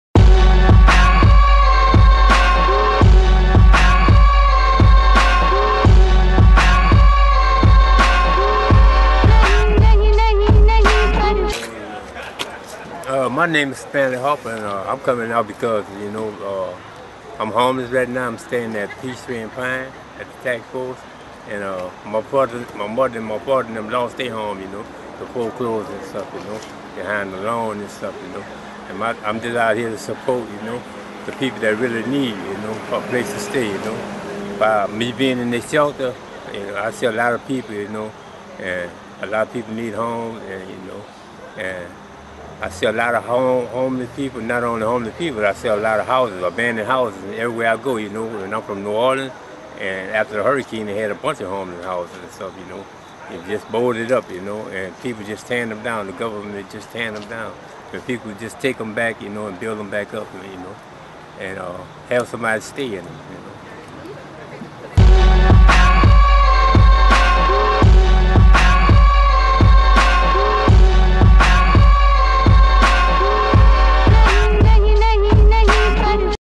The People vs Fannie and Freddie: Testify!
People describe why they're going to Atlanta's regional action against Fannie Mae and Freddie Mac.